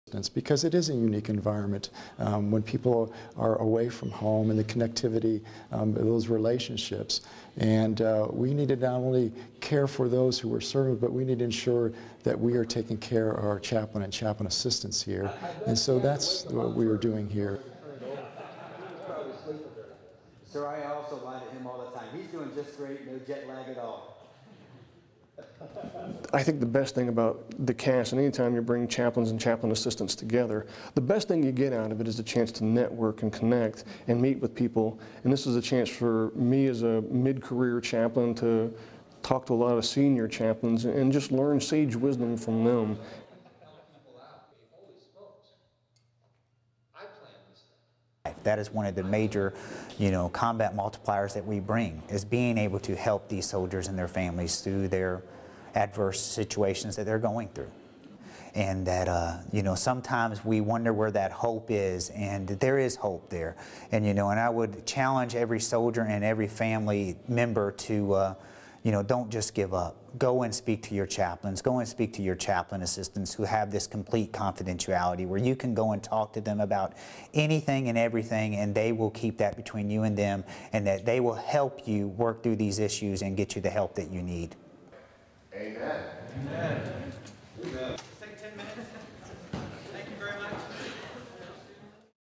US Army Europe CAST-E Training Conference 2012
Chaplains from around U.S. Army Europe gathered this week for the Chaplaincy Annual Sustainment Training-Europe, a week-long event designed to conduct professional development training and provide relevant and ready ministry teams in support of future pastoral operations.